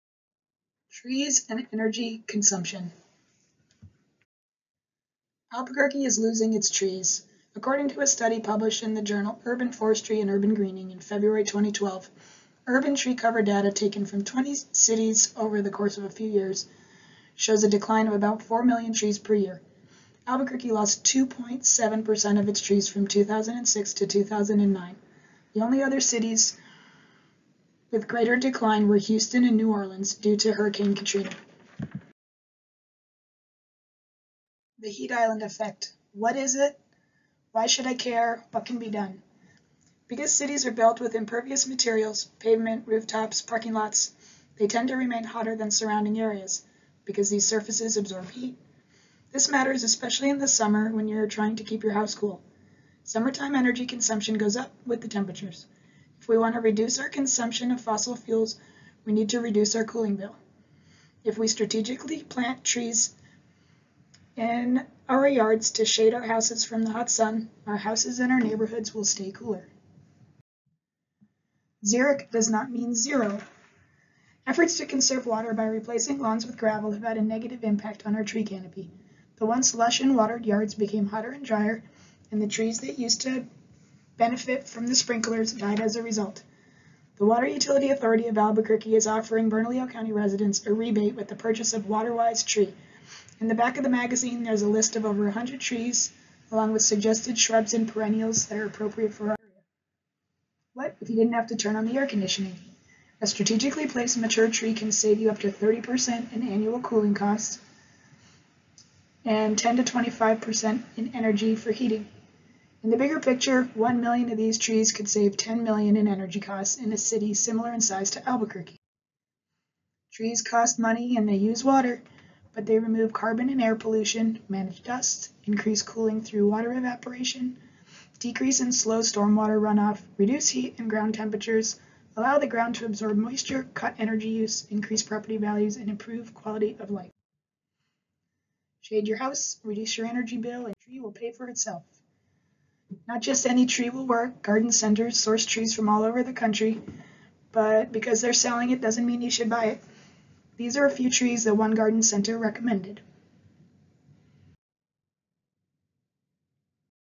'Trees and Energy Comsumption' by Julie Winsor
INTRO: The topic of my presentation is ‘Energy Consumption and Trees’. I will talk about how Albuquerque’s declining tree canopy and an increase in impervious surfaces are directly related to the Heat Island Effect and resource depletion.

OBJECTIVE: My goal is to inform my peers that previous city incentives to reduce household water usage increased water consumption. In years past, residents planted non-native species and watered as we rested atop a bottomless aquifer. There was a change in consciousness and homeowners were given water credit if they removed lawns and spread gravel, the 'xeric' landscape. The pre-existing trees no longer got water because they were now surrounded by hot rocks, got stressed and declined. Tree loss means less shade and hotter neighborhoods. When it is hot, we crank up the AC. This uses energy. People fear to plant or maintaining trees will be expensive. NOT true. Trees allow the ground to absorb moisture, reducing other issues, flash floods, and erosion. They assist in groundwater recharge, sequester carbon and make places more hospitable. If I can convince 10 people to plant a tree, a native shade tree, I feel I have accomplished something positive.

METHODS: I will create a PowerPoint with narration. I have never added audio but there is a YouTube video for everything.  I have photos and images of appropriate tree selections.

RESULTS: I am pleased with my progress and the flow of this project. The design prompts help keep me on point. My weakness is the tech aspect. This may prove to be the most difficult part.

CONCLUSION: The most apparent obstacle I now see is not convincing residents to plant the trees, it is the demographic disparities that prevent this from happening. If you can’t afford a shovel or a hose how can you afford a tree?